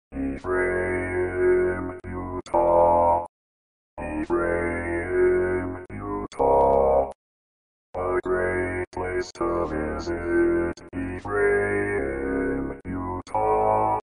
How to Say or Pronounce USA Cities — Ephraim, Utah
This video shows you how to say or pronounce Ephraim, Utah.

A computer said Ephraim, Utah. How would you say Ephraim, Utah?